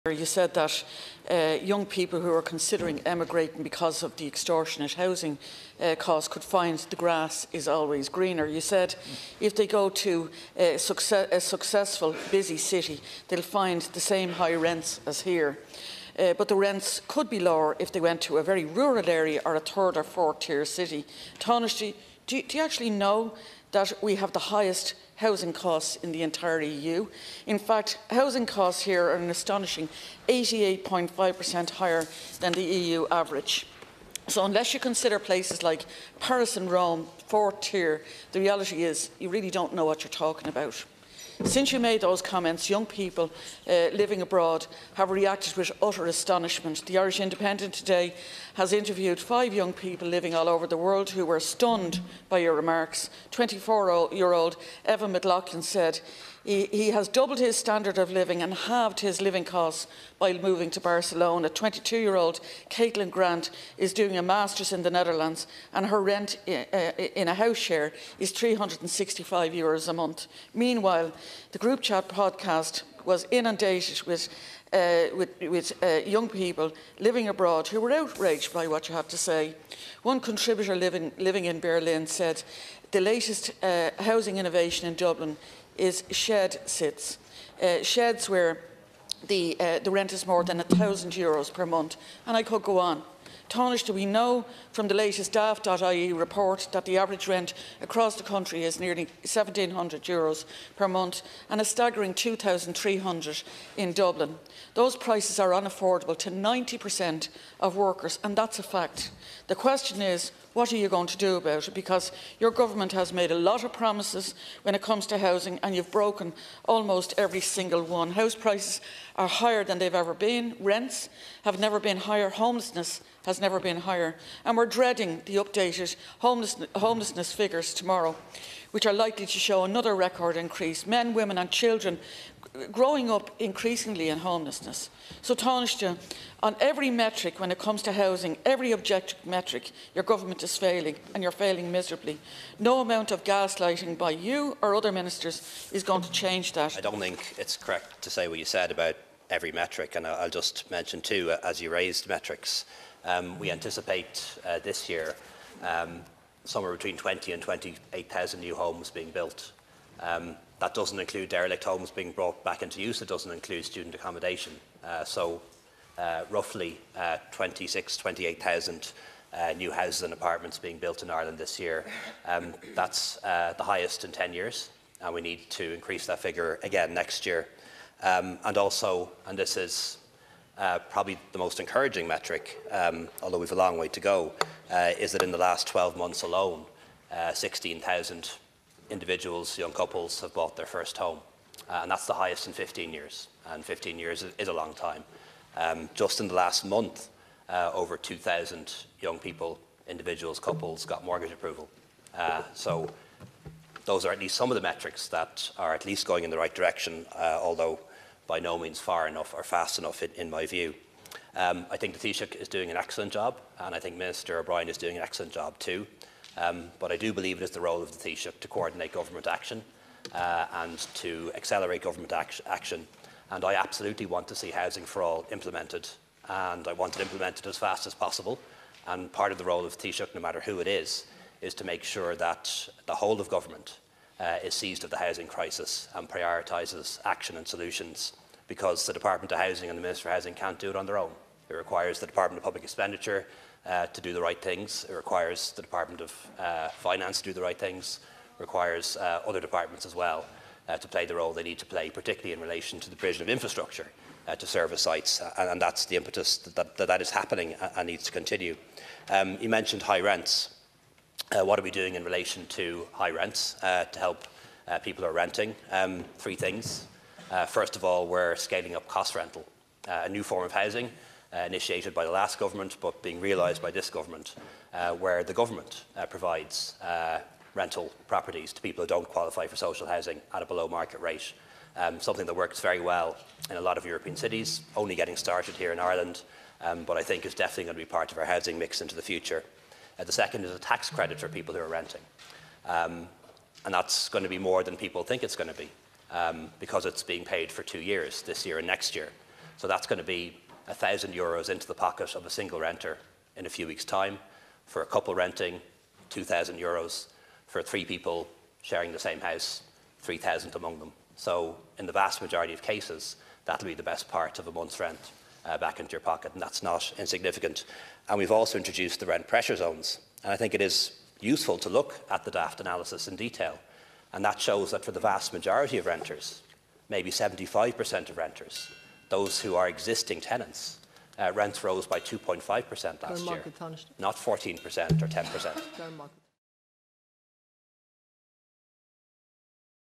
'You don't have a clue' – Varadkar under fire for suggesting rent not lower for people overseas
Social Democrats' Catherine Murphy questioned Tánaiste Leo Varadkar over comments around rent rates abroad.